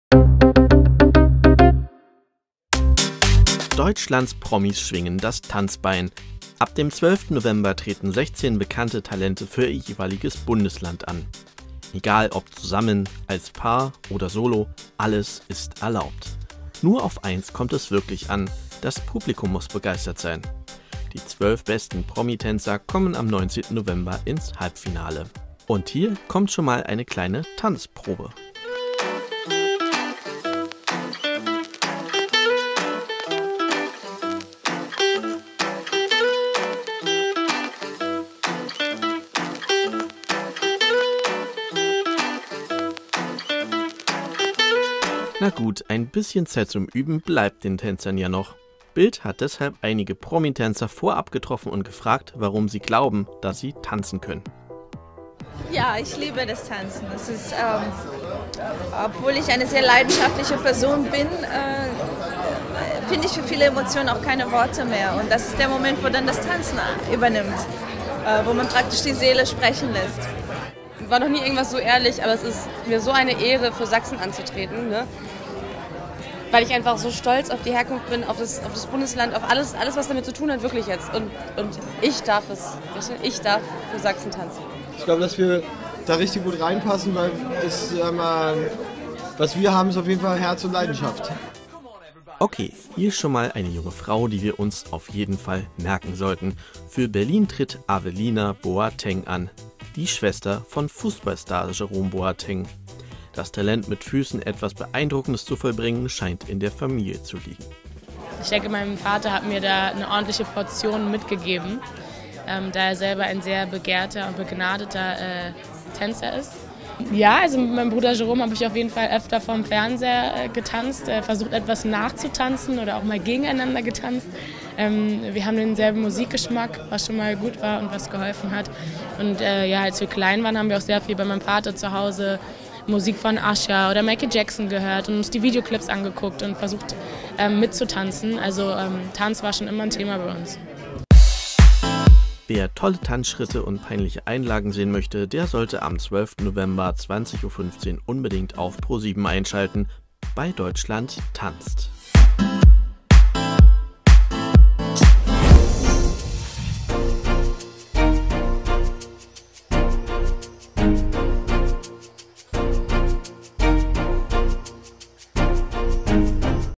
Kusmagk, Brandao, Boatengs Schwester: Deutschland tanzt - Welches Bundesland gewinnt?
Am 12. November startet 20:15 Uhr auf Pro7 die neue Sendung "Deutschland tanzt. BILD spricht mit einigen Kandidaten.